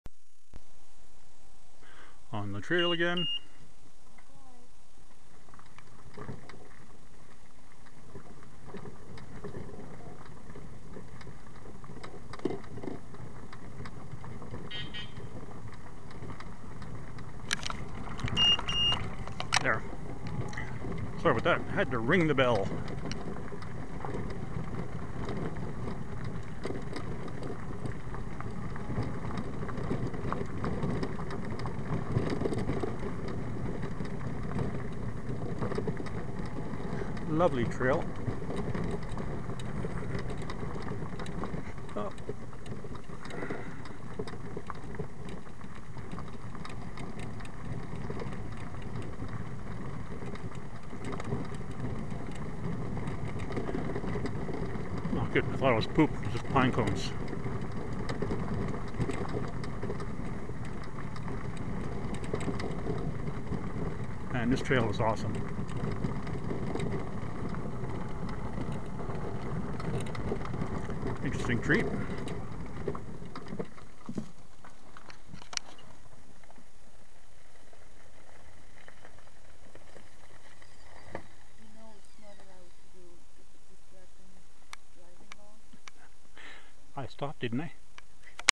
0005 Jarvis Lake Tine
On the trail around Jarvis Lake. No bears so far.